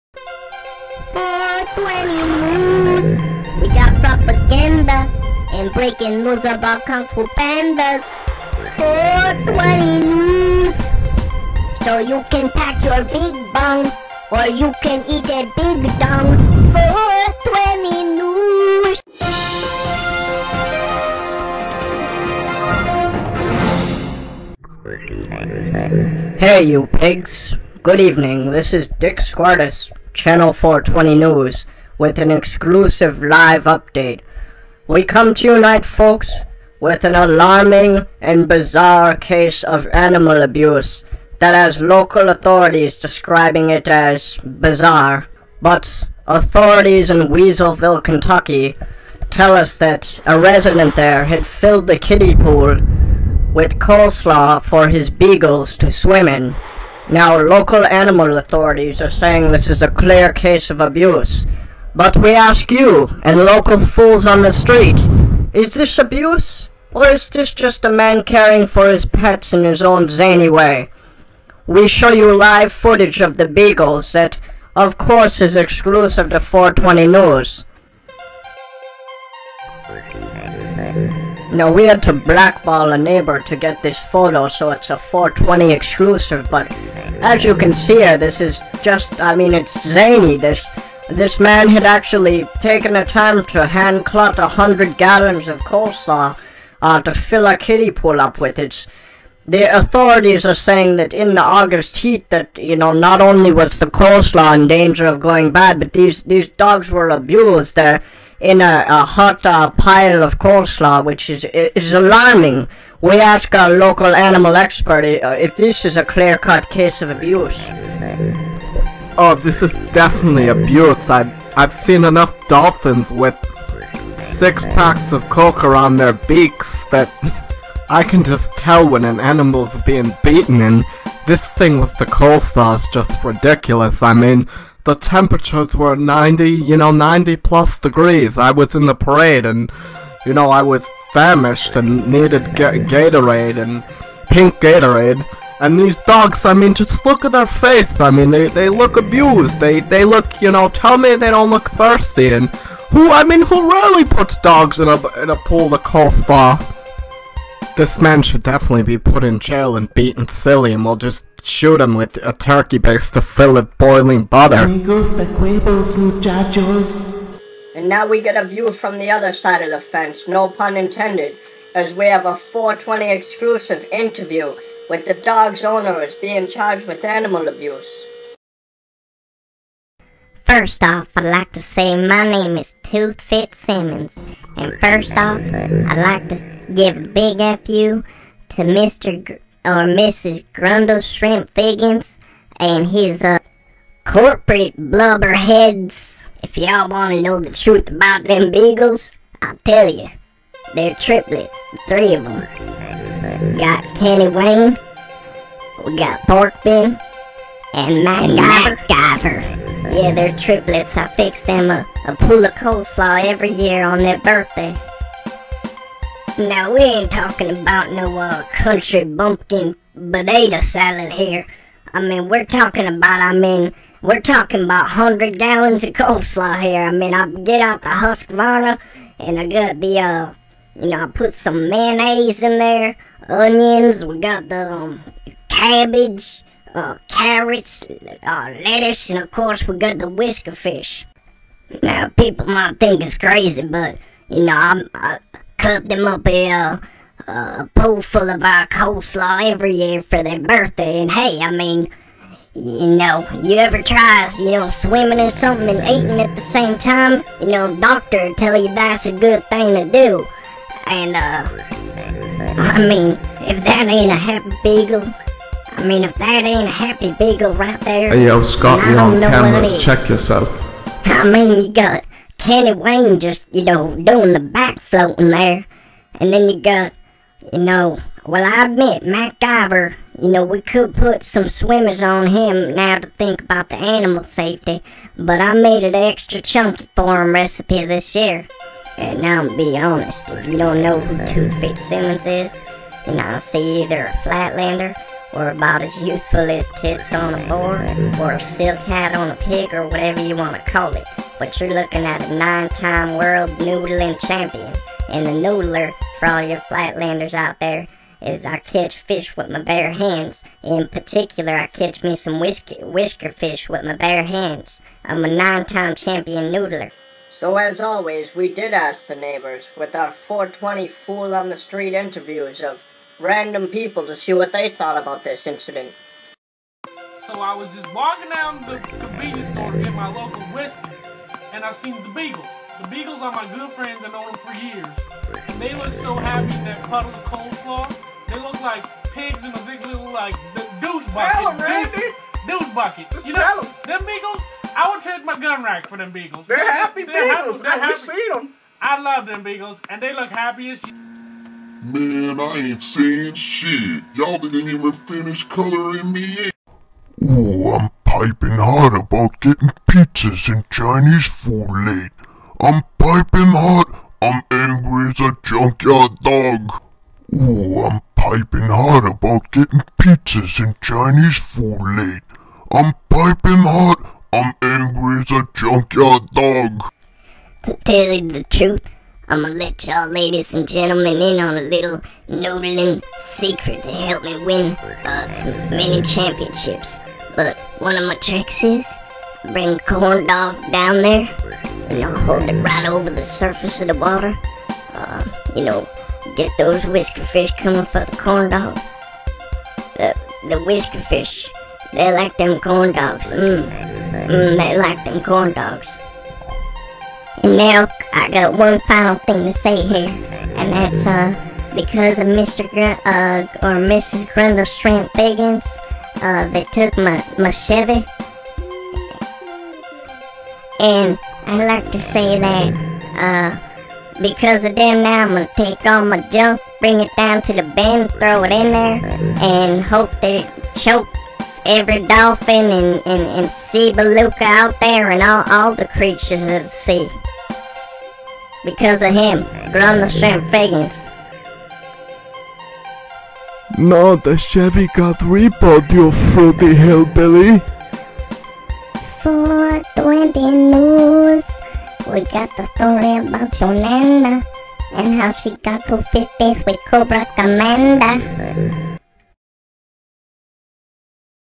Beagles In Coleslaw
420 news exclusive.  Feat. Dick Squyrtis award winning anchor.  Live Update ! Beagle Massacre in Weaselville KY sets off animal rights showdown  (Repeat)